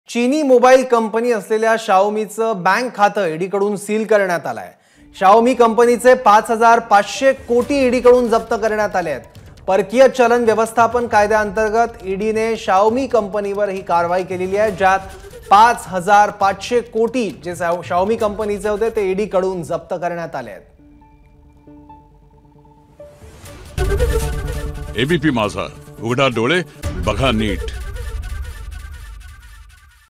चिनी मोबाईल कंपनी Xiaomi चं खातं ED कडून सील, 5 हजार 500 कोटी ईडीकडून जप्त : ABP Majha
चिनी मोबाईल कंपनी  Xiaomi चं cखातं ED कडून सील, 5 हजार 500 कोटी ईडीकडून जप्त : ABP Majha